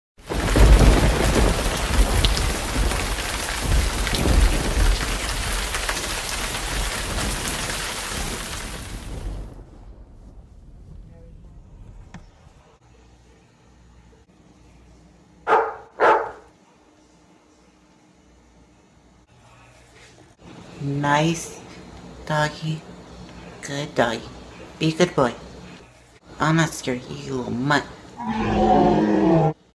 The Adventures Of Leo Ep. 1 "Beware Of Dog"
Witness Leo the Lion being pitted against his worst enemy, man's best friend.